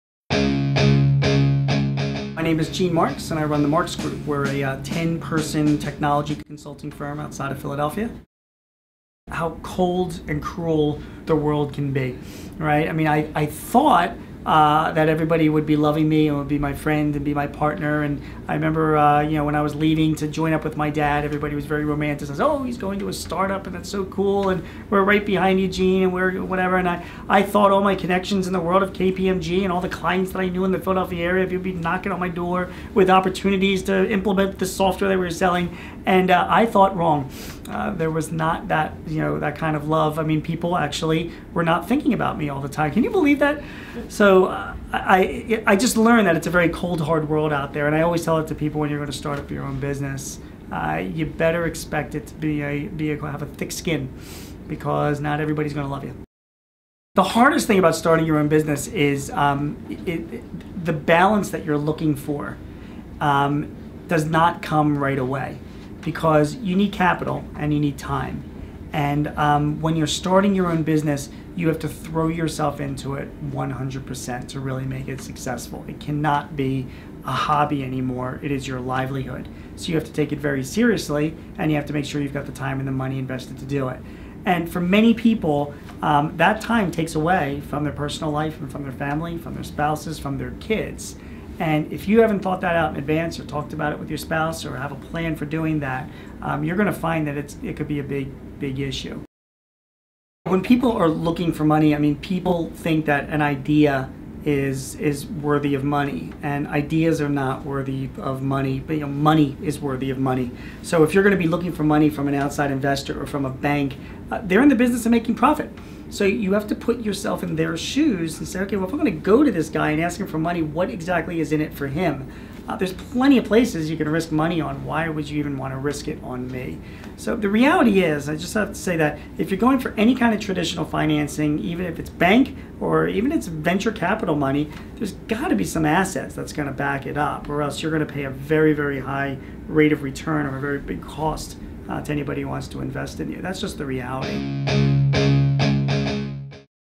Hiscox Authors Series with Gene Marks: The Hardest Part About Starting a Business
Hiscox presents the Authors Series for Entrepreneurs for small business owners. Gene Marks is a small business columnist for The New York Times, owner of the Marks Group and author of 'In God We Trust, All Others Pay Cash — Simple Lessons from Smart Business People.' In this video, Gene shares the hardest part about starting a business.